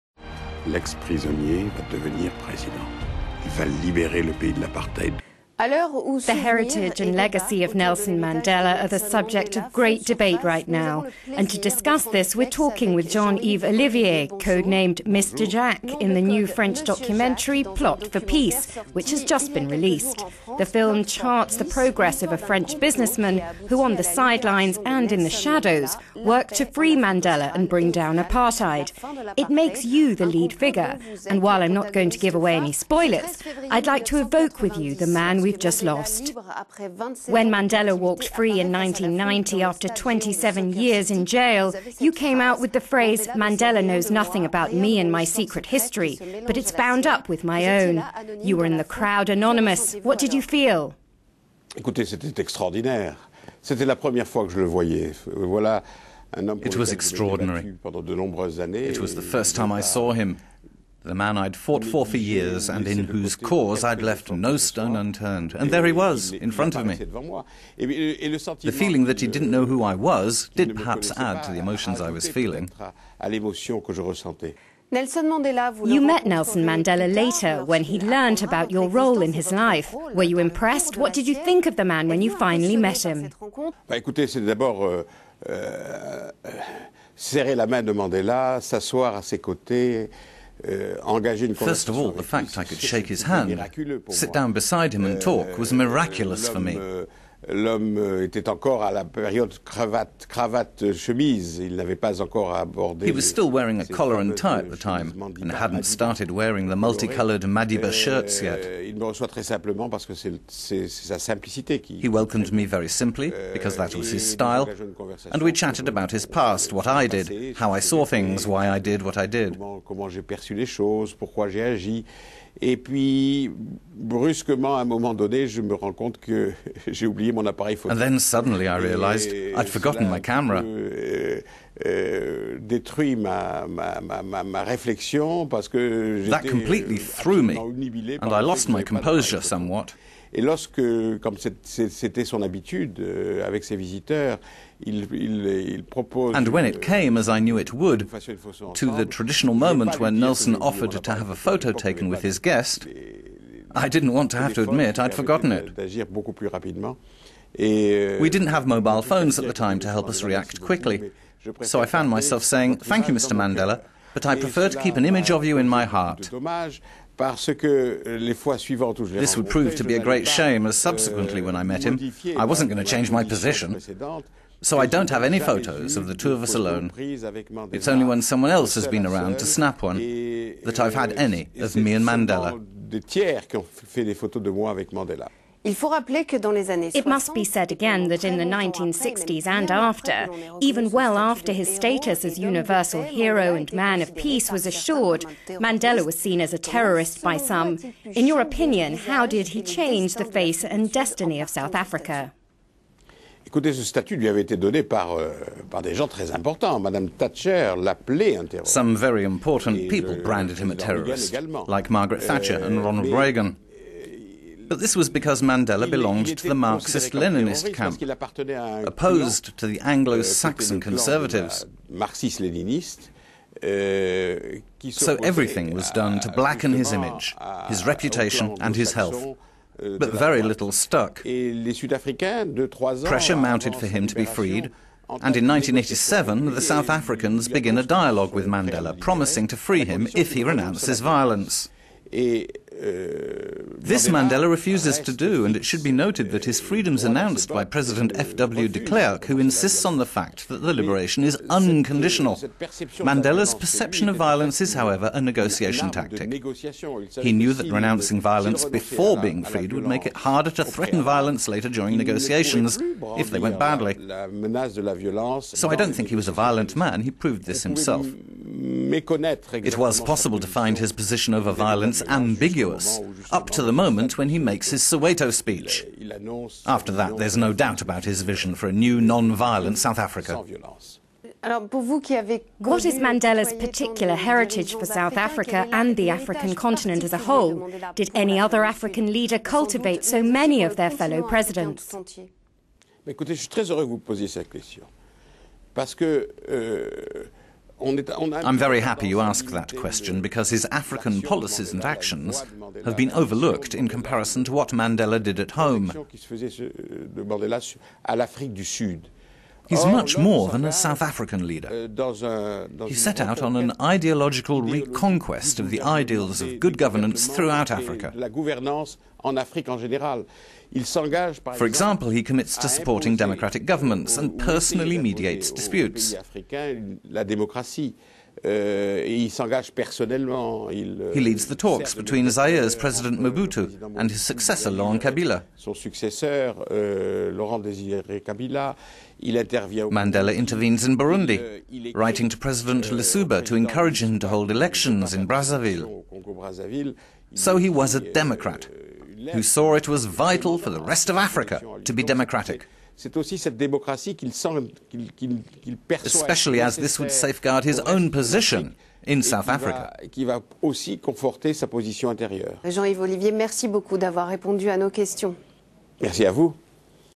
Shadowy French figure in the Nelson Mandela story
The heritage and legacy of Nelson Mandela are the subject of great debate right now, and to discuss...

The heritage and legacy of Nelson Mandela are the subject of great debate right now, and to discuss this euronews talked to Jean Yves Ollivier, codename "Mr Jacques" in the new French documentary  "Plot for Peace", which has just had its London premiere.

Sophie Desjardin, euronews:

"The film charts the progress of a French businessman who, on the sidelines and in the shadows, worked to free Mandela and bring down apartheid. It makes you the lead figure, and while I'm not going to give away spoilers, I'd like to evoke with you the man we have just lost.
On February 13 1990 when Mandela walked free after 27 years in jail and appeared in front of a crowd in Soccer City you came out with this phrase - 'Mandela knows nothing about me and my secret history, but it's bound up with his own'. You were there in the crowd, anonymous. What did you feel?"

Jean Yves Ollivier:

"It was extraordinary. It was the first time I saw him, the man I'd fought for for years and in whose cause I'd left no stone unturned was there, in front of me. The feeling that he didn't know who I was did perhaps add to the emotions I was feeling."

Sophie Desjardin:

"Nelson Mandela met you later, when he learned about your existence and your role in his life. Were you impressed by the meeting? What did you think of the man when you finally met him?"

Jean Yves Ollivier:

"First of all the fact I could shake his hand, stand beside him and talk was miraculous for me. He was still wearing a collar and tie at the time and hadn't started wearing the multicoloured Madiba shirts yet. The received me very simply, because that was his style, and we chatted about his past, what I did, how I saw things, why I did what I did.
And then suddenly I realised I had forgotten my camera. That completely threw me, and I lost my composure somewhat. And when it came, as I knew it would, to the traditional moment when Nelson offered to have a photo taken with his guest I didn't want to have to admit I'd forgotten it.
We didn't have mobile phones at the time to help us react quickly, so I found myself saying 'Thank you Mr. Mandela but I prefer to keep an image of you in my heart.'
This would prove to be a great shame as subsequently when I met him I wasn't going to change my position, so I don't have any photos of the two of us alone. It's only when someone else has been around to snap one that I've had any of me and Mandela."

Sophie Desjardin:

"It must be repeated that in the 1960s and well beyond, even well after his status as universal hero and man of peace was accorded,  Mandela was seen as a terrorist by some. In your opinion how did he change the face and destiny of South Africa? "

Jean Yves Ollivier:

"Some very important people branded him a terrorist, like Margaret Thatcher and Ronald Reagan, but this was because Mandela belonged to the Marxist-Leninist camp, opposed to the Anglo-Saxon Conservatives.
So everything was done to blacken his image, his reputation, and his health, but very little stuck. Pressure mounted for him to be freed and in 1987 the South Africans begin a dialogue with Mandela, promising to free him if he renounces violence.
This Mandela refuses to do. And it should be noted that his freedom is announced by President FW de Klerk, who insists on the fact that the liberation is unconditional.
Mandela's perception of violence is, however, a negotiation tactic. He knew that renouncing violence before being freed would make it harder to threaten violence later during negotiations, if they went badly.  So I don't think he was a violent man, he proved this himself.  It was possible to find his position over violence ambiguous up to the moment when he makes his Soweto speech. After that there's no doubt about his vision for a new, non-violent South Africa."

Sophie Desjardin:

"How did the man that you knew manage to shed the skin of an African Nationalist who dreamed of throwing the white colonist back in the sea, into the emblem of heroic reconciliation that we celebrate today?"

Jean Yves Ollivier:

"First of all it's because of his stature as leader. Don't forget that at the end of the 1970s the ANC slogan is 'Death to Apartheid', and this is gradually replaced, in large part thanks to Winnie Mandela, by the far more positive 'Free Mandela'.